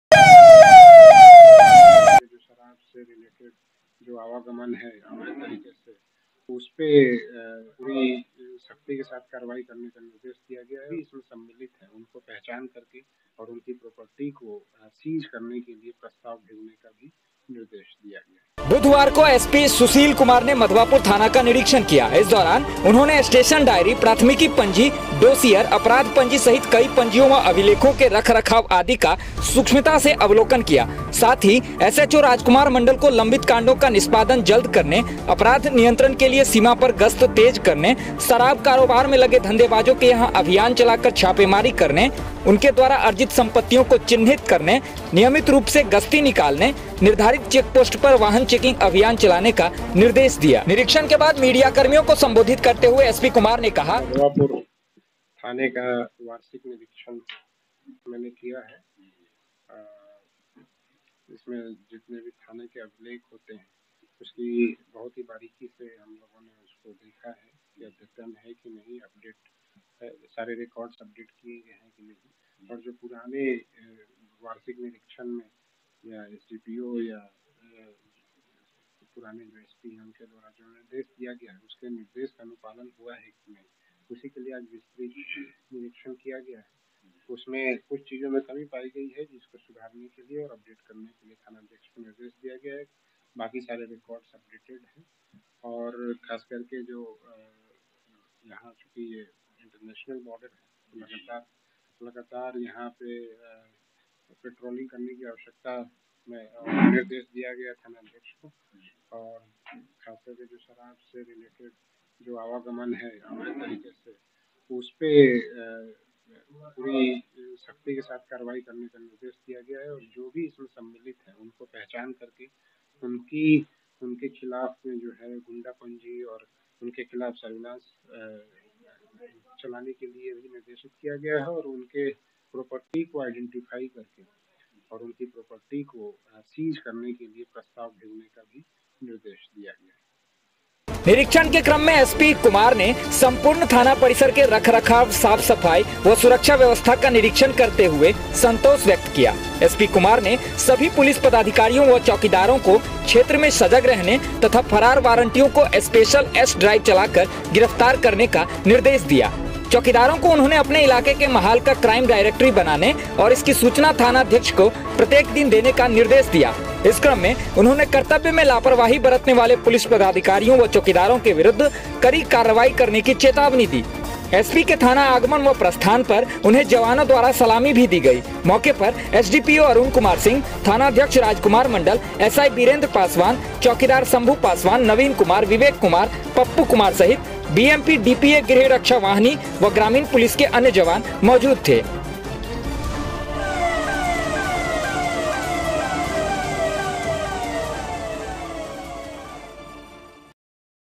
शराब कारोबारियों द्वारा अर्जित संपत्तियों को चिन्हित कर दें रिपोर्ट, होगी जब्त : SP Madhubani
madhubani SP सुशील कुमार ने किया मधवापुर थाना का निरीक्षण, दिए कई निर्देश।